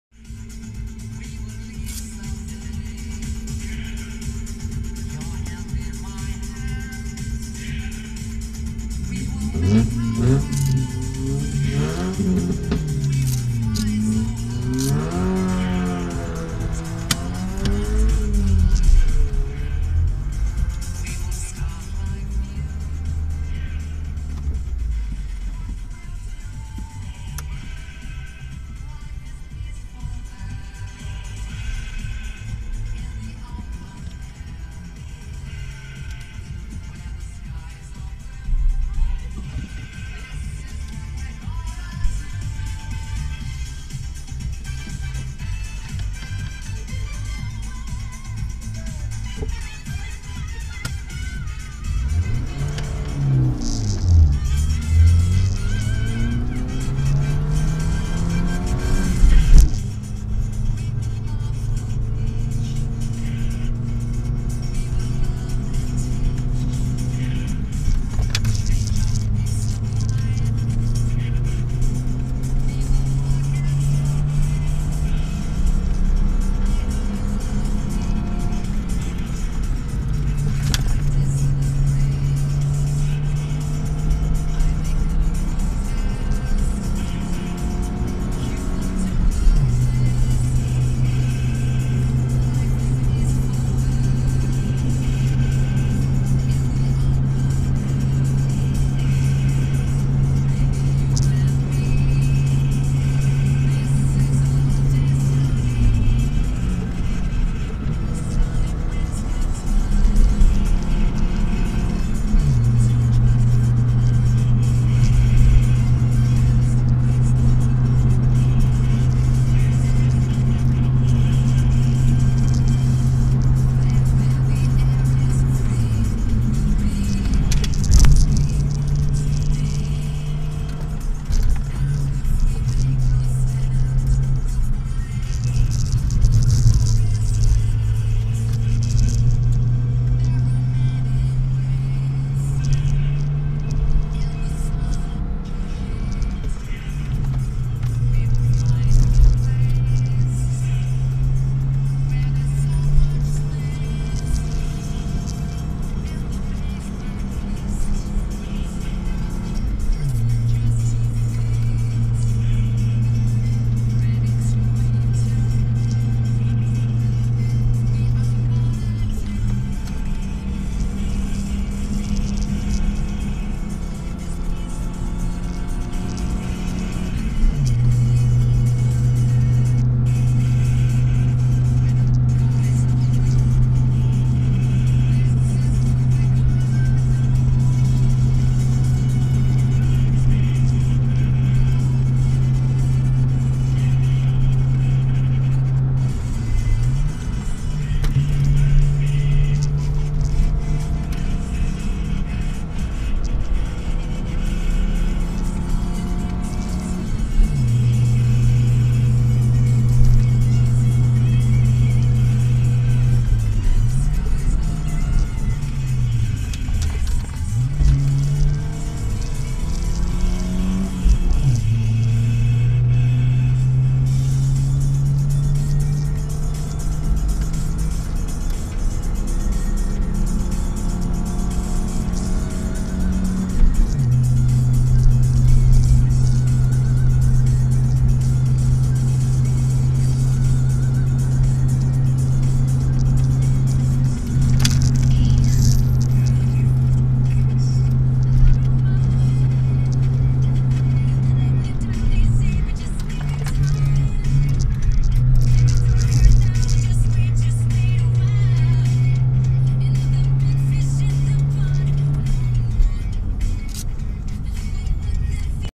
Krušnohorský pohár Krupka 2017 - sobotní první ostrá jízda
Krušnohorský pohár Krupka - Komáří Vížka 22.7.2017 - sobotní první ostrá jízda
Ford Fiesta 1,3